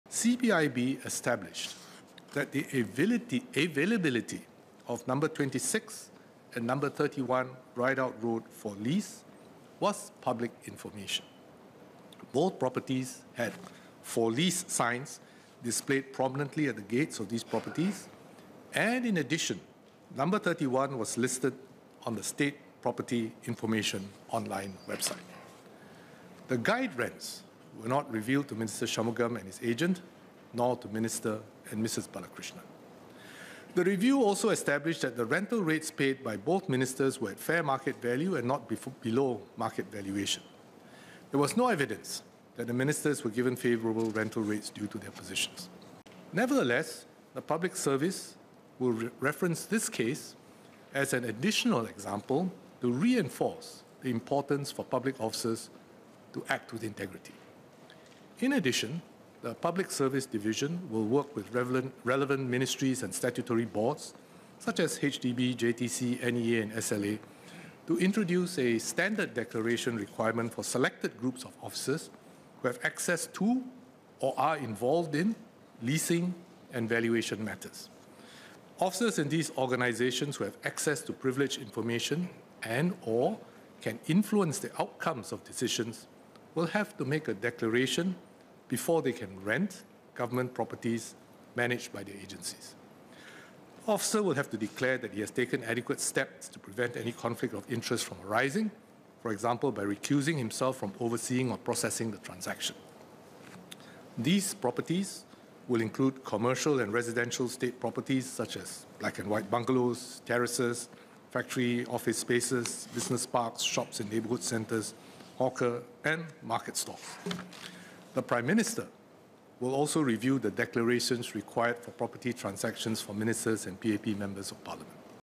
New standard declaration for Singapore public officers with access to privileged info: Teo Chee Hean
Officers in government ministries and statutory boards with access to privileged information and influence on decisions will soon have to make a declaration before they can rent government properties managed by their agencies, Senior Minister Teo Chee Hean told parliament on Monday (Jul 3). The Public Service Division will work with the ministries and statutory boards to introduce a standard declaration requirement for selected groups of officers who have access to, or are involved in leasing and valuation matters.

Mr Teo announced this as part of a ministerial statement on the rental of state properties on Ridout Road to Ministers K Shanmugam and Vivian Balakrishnan, where he reiterated that official investigations found that the ministers and public officers as well as private sector intermediaries "conducted themselves properly". "Nevertheless, the public service will reference this case as an additional example to reinforce the importance for public officers to act with integrity," Mr Teo said.